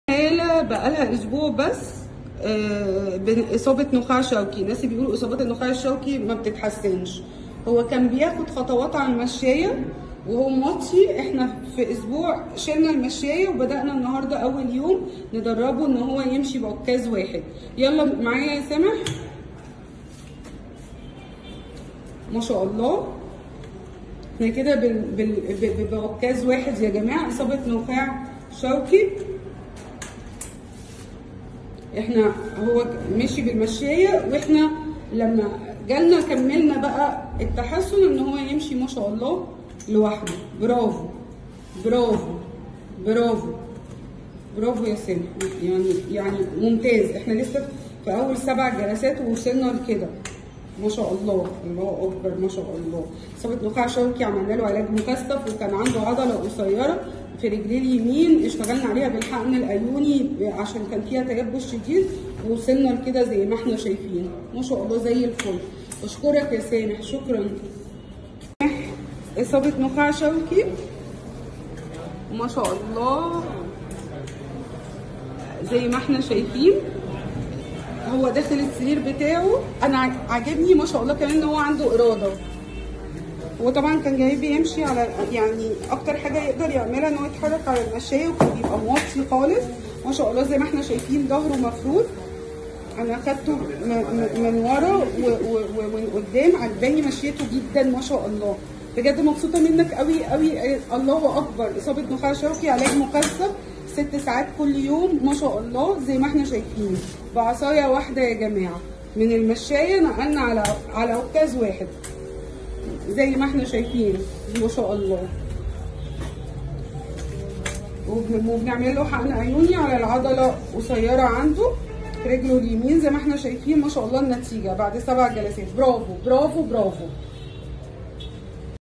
حالة اصابة نخاع شوكي بعد اسبوع يمشي بعكاز واحد فقط مع د / رانيا السيد عبد العليم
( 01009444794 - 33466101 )
عيادة المنيل : ش المنيل ميدان الباشا فوق الدمياطي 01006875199